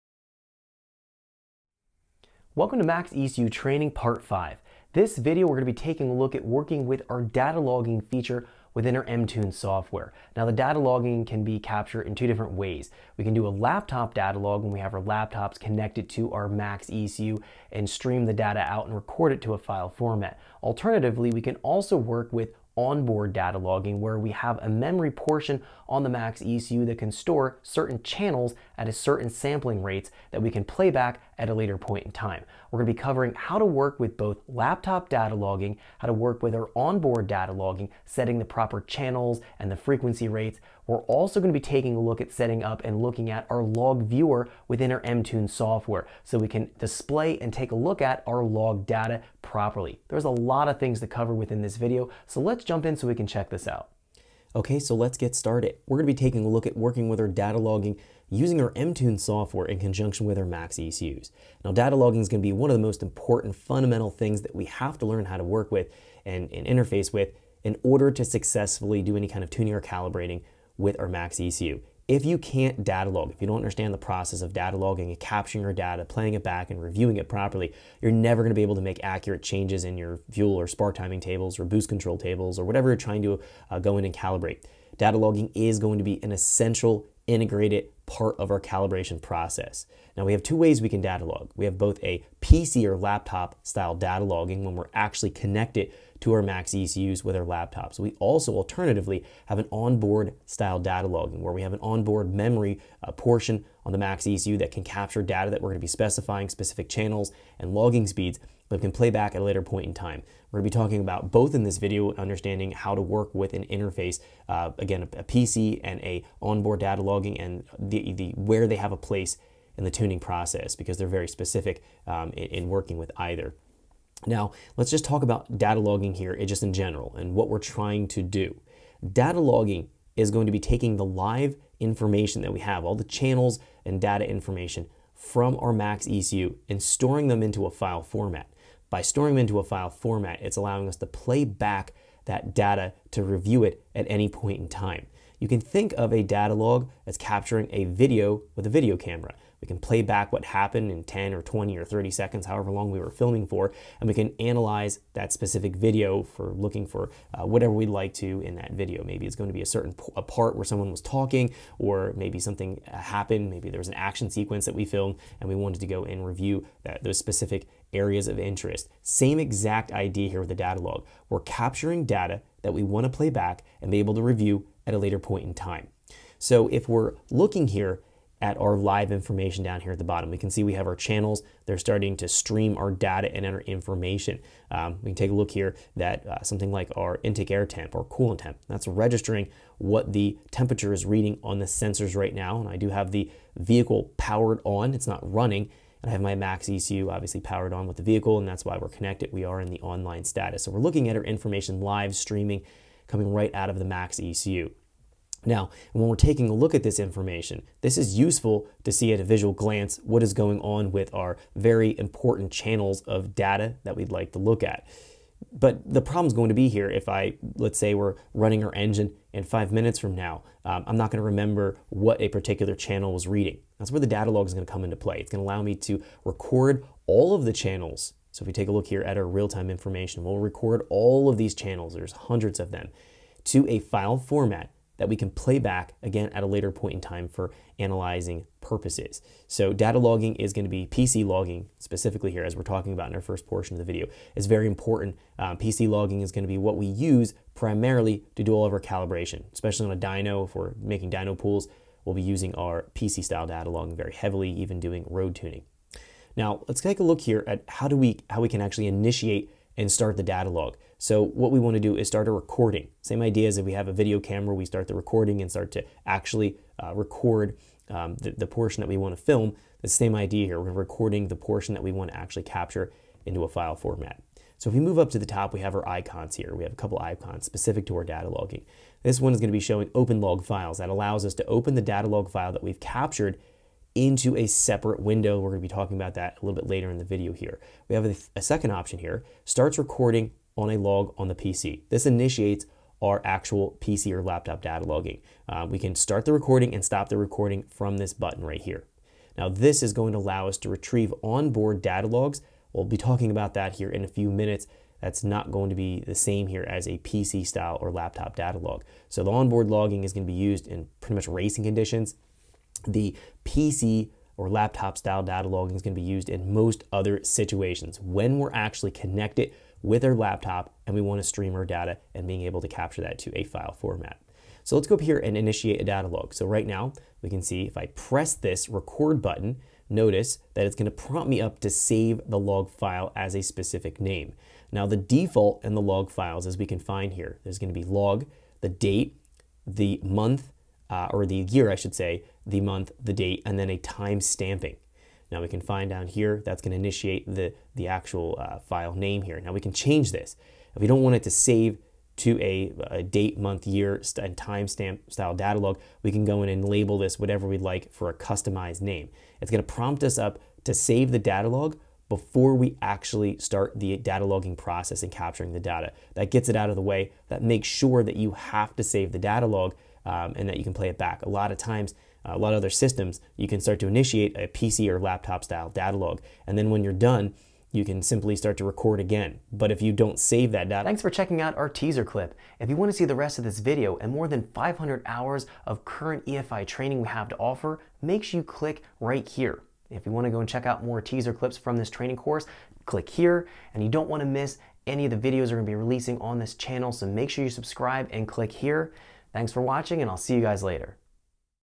MaxxEcu Training Part 5: Datalogging | Evans Performance Academy
MaxxEcu Training Part 5: Datalogging walks you through how to properly work with both laptop and on-board data logging using the Mtune software.  In addition, we explore how to utilize the log viewer software to analyze data.